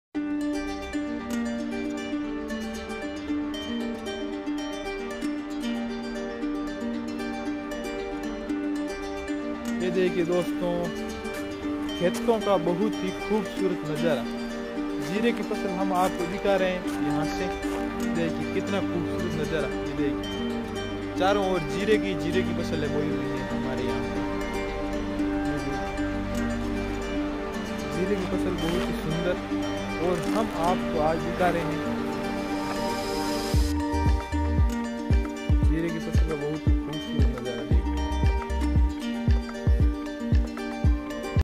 खेत का बहुत ही खूबसूरत नजारा // village life vlogs //
नमस्कार दोस्तों स्वागत है मेरी यूट्यूब चैनल में आज मैं आपको दिखा रहा हूं खेत का बहुत ही खूबसूरत नजारा तो वीडियो को ज्यादा से ज्यादा अपने और दोस्तों के साथ शेयर करें और मैं आज आपको दिखा रहा हूं जीरे की फसल का बहुत ही सुंदर नजारा//